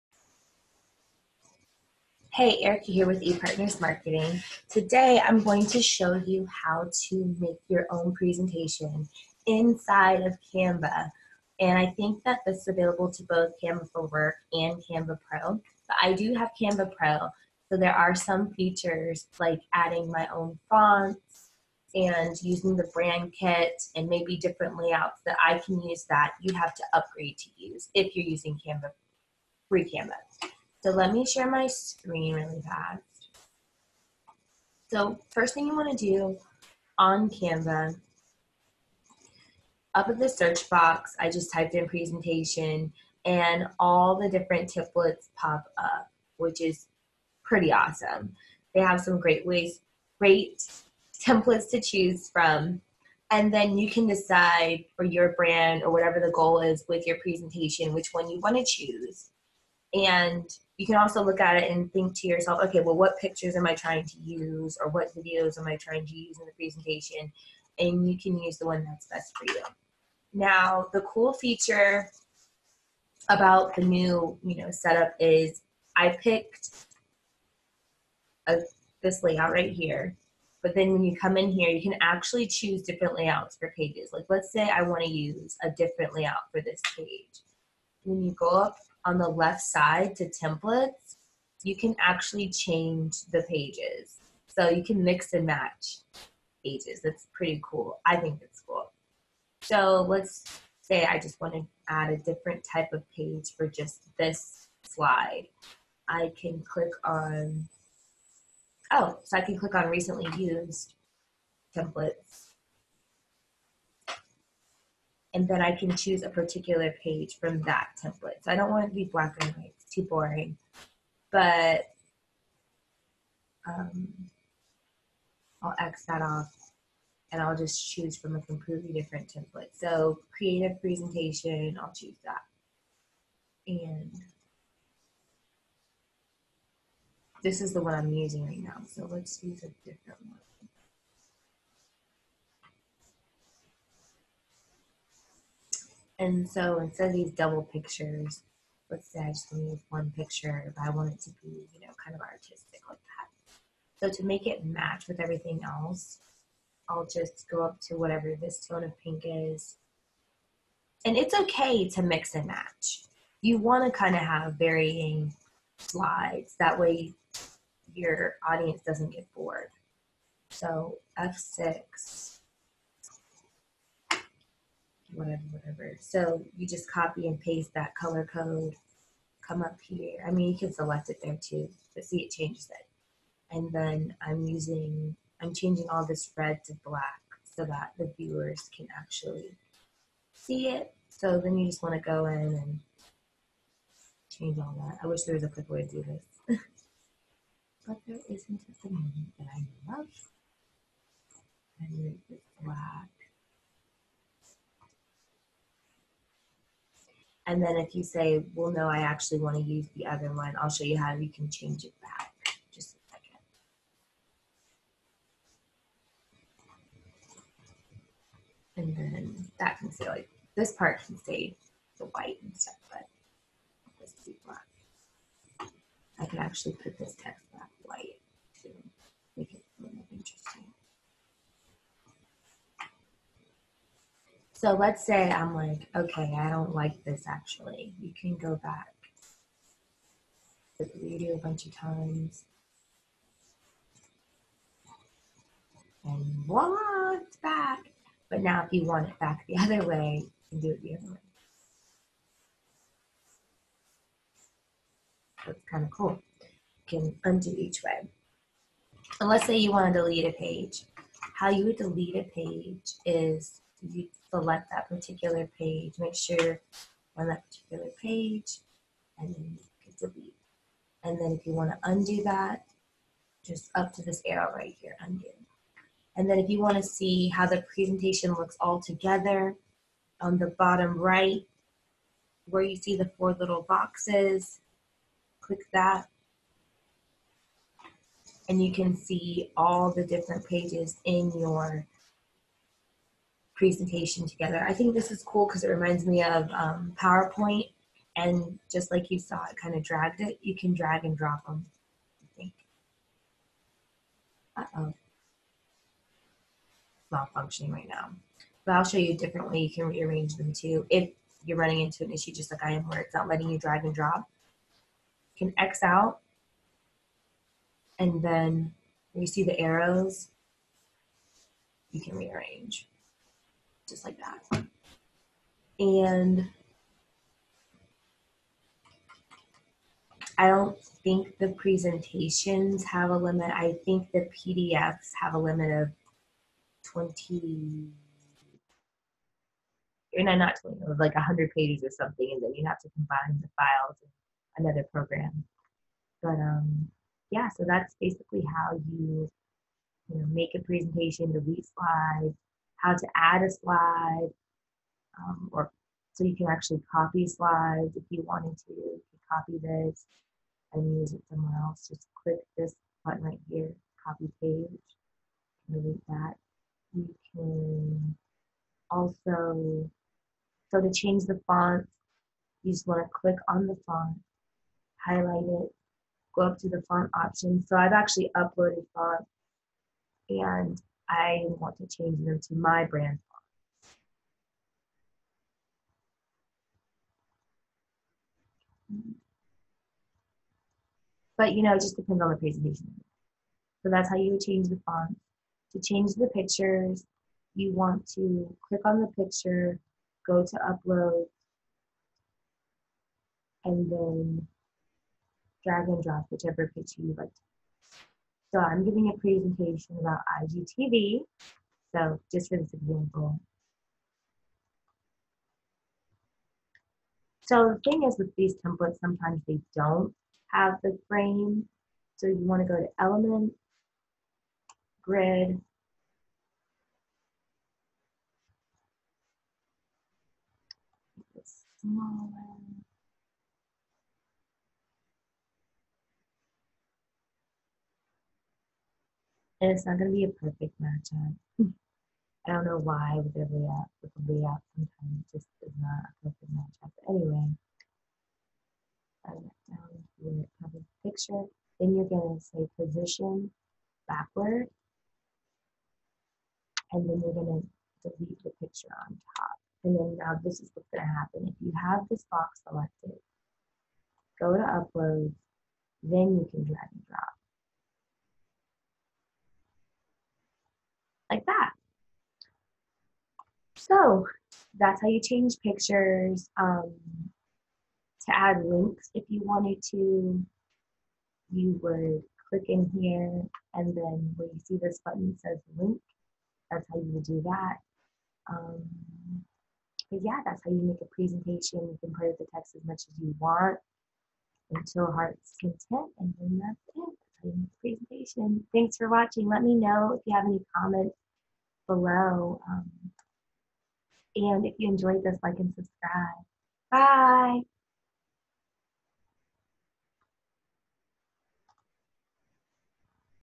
HOW TO MAKE A SLIDE DECK | How to Make a Presentation with CANVA
Got a big presentation coming up and have no time to mess around with PowerPoint or Google Slides? Check out how to easily make a slide deck (presentation) in Canva!  !